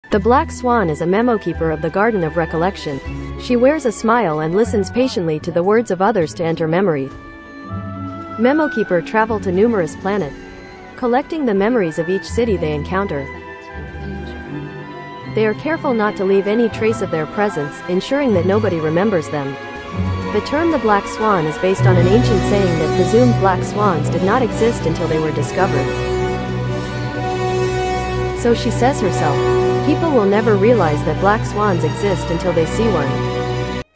Black Swan explained - Honkai Star Rail
What exactly is the Black Swan in Honkai Star Rail? What is her lore? What is this character's whole purpose??? Let's see...